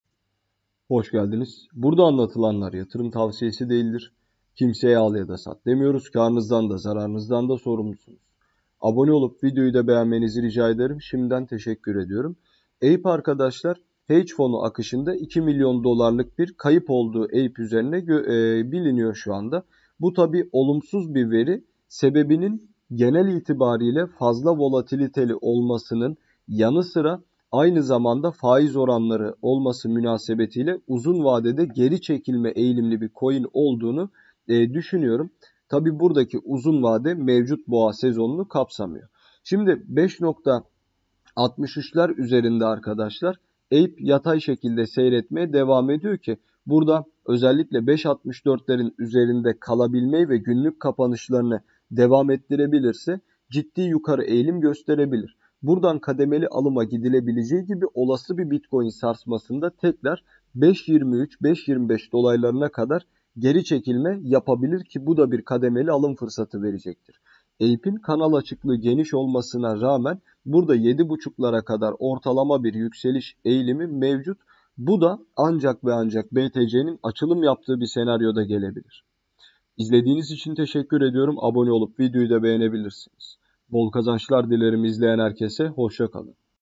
YANILMAYAN ŞİRKET:"APECOİN, KISA SÜRE İÇİNDEE BATACAK!"🚨"APE FİYATI BARİZ ŞEKİLDE İFLASA GİDİYOR!!"🚨
Bu videomuzda kripto borsalarında oldukça popüler bir coin olan APE coin'i inceledik. Ape coin'in destek ve direnç noktalarını belirterek kısa, orta ve uzun vade hedeflerinden söz ettik. Ape alım ve satım noktalarını belirleyerek, hangi noktadan almalıyız? Gibi sorulara yanıt olmaya çalıştık. Ape, uzun vadede yakaladığı popülerite ve beklenen hacimle Binance'ın da olumlu yaptırımlarıyla 2022'de çok büyük yükselişler yapabilir. Ape anlık 16.8$ seviyelerinde ve Ethereum ve web3 farkıyla 80$ seviyelerine kadar 2022'de yükselebilir. Ape son gelişmelerle birlikte Mana coin'i geçerek piyasa değeri bakımından en yüksek metaverse projesi oldu. Bu videoda yatırım tavsiyesi verilmemiştir.